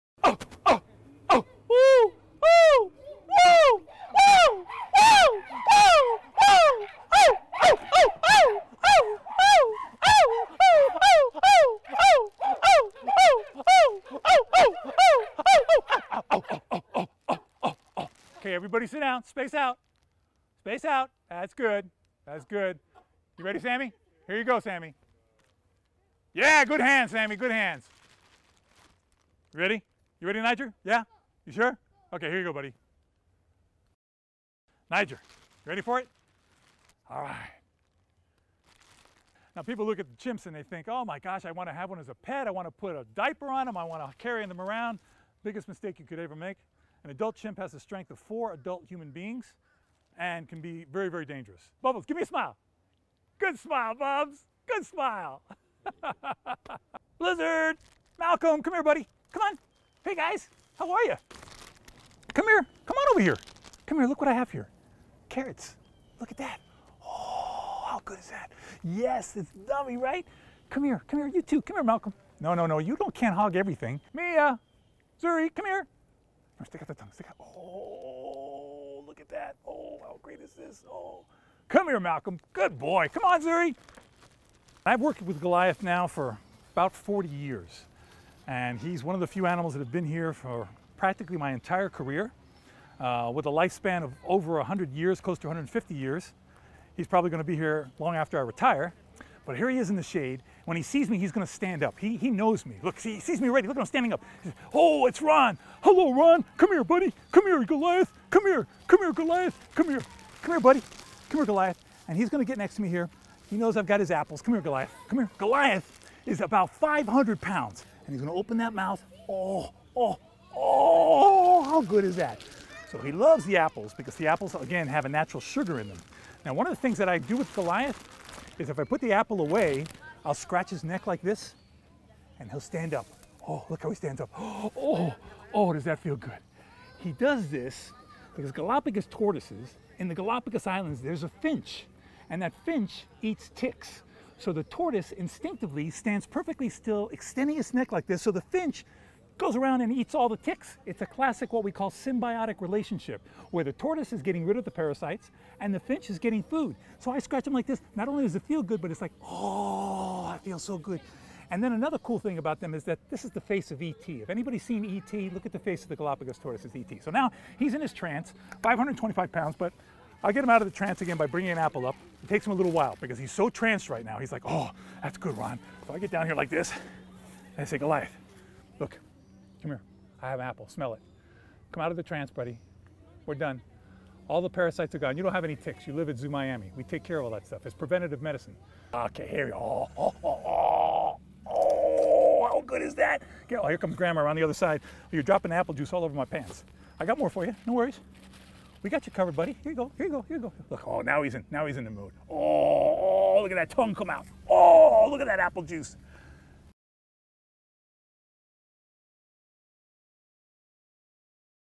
Watch Ron Magill Feed Animals at Zoo Miami
Zoo Miami's communications director, ambassador, and animal expert Ron Magill and Zoo Miami will both soon celebrate 40 years in Kendall.

Video by Al Diaz / Miami Herald